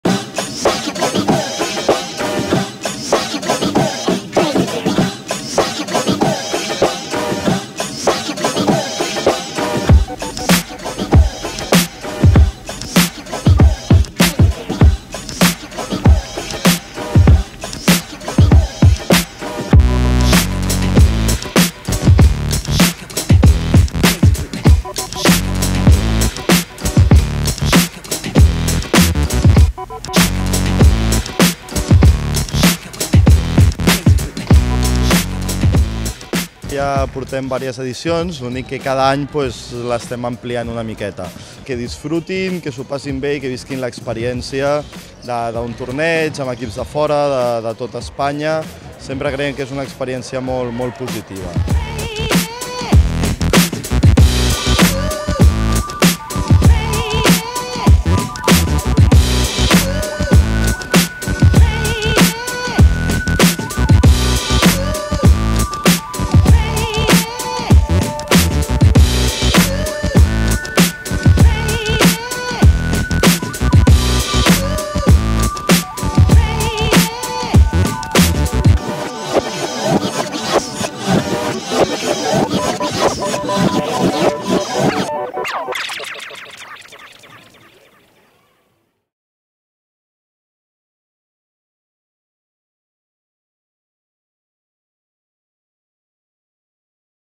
Un cap de setmana de bàsquet
Durant tot el cap de setmana se celebra a Atrium i Podium el Torneig de Bàsquet Infantil Ciutat de Viladecans Unilever. 22 equips infantils arribats d'arreu d'Espanya competiran durant tres dies.

Enguany s'incorpora una divisó masculina. El FC Barcelona, el Real Madrid, el C. O. Manresa o el Joventut de Badalona són alguns dels participants.